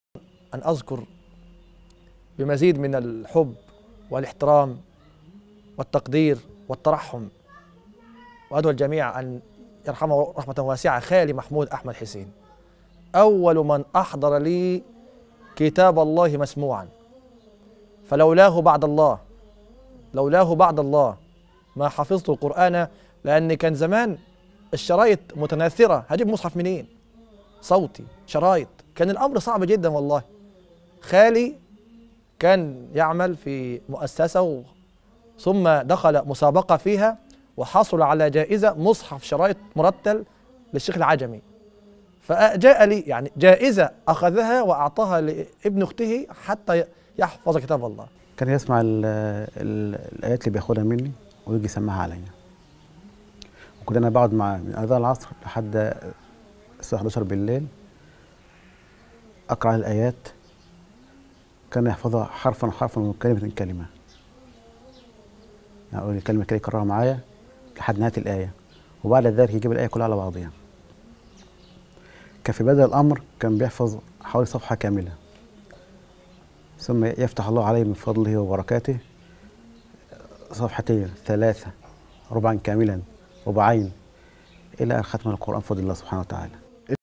أعظم هدية جاءت لي من خالي "رحمه الله" وبسببه حفظت القرآن كاملاً .. وهذا ما حصل!
الحلقة الثالثة
الخميس 9:30م

نسعد بخدمتكم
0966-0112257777